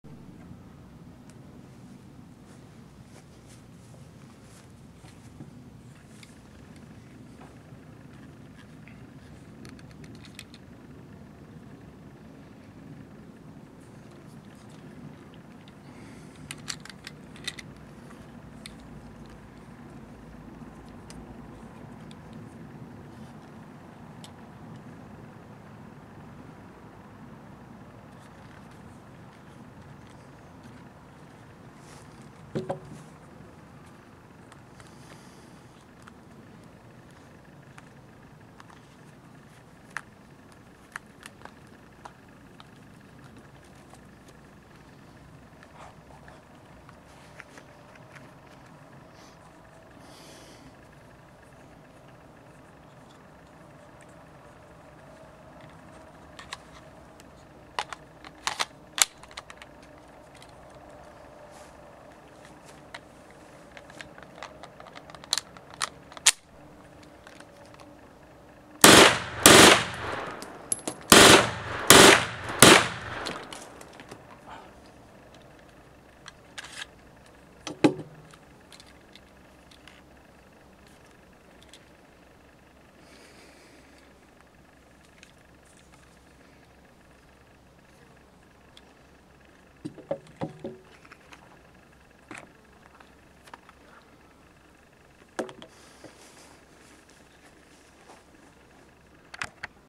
Kalaschnikow Luger - Full Auto Test No. 2
Now I am able to show that it is possible to invent an interchangeable full auto selector for Luger pistols or better Luger Carbines.